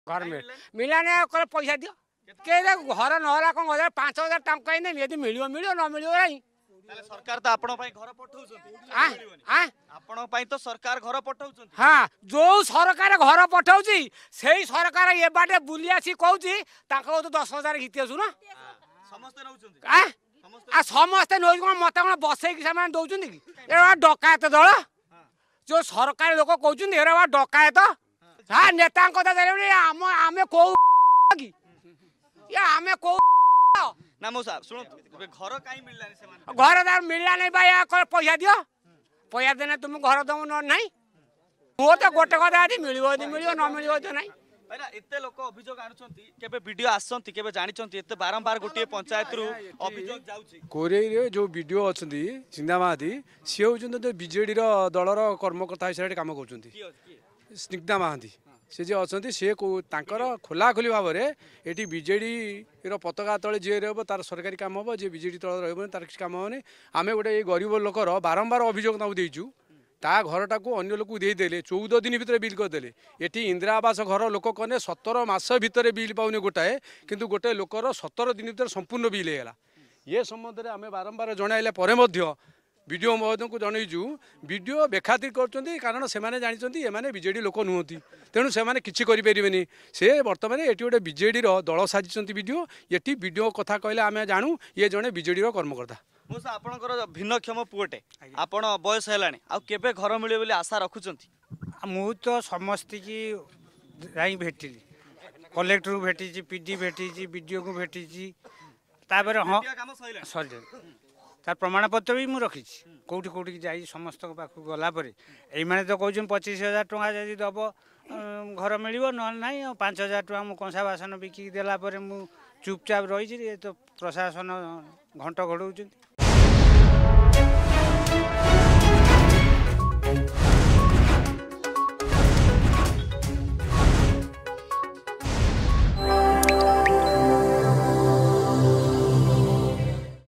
ଯାଜପୁର କୋରେଇ ଅଞ୍ଚଳରେ ଆବାସ ଦୁର୍ନିତୀ ଘଟଣା
ଯାଜପୁର କୋରେଇ ଅଞ୍ଚଳରେ ଆବାସ ଦୁର୍ନିତୀ ଘଟଣା : ଯେଉଁ ସରକାର ଘର ଦେଉଛନ୍ତି ସେଇ ସରକାର ପଛପଟେ 10 ହଜାର ମାଗୁଛନ୍ତି...
ଅଧିକ ଖବର ପାଇଁ ଡ଼ାଉନଲୋଡ କରନ୍ତୁ “Argus News” ଆପ୍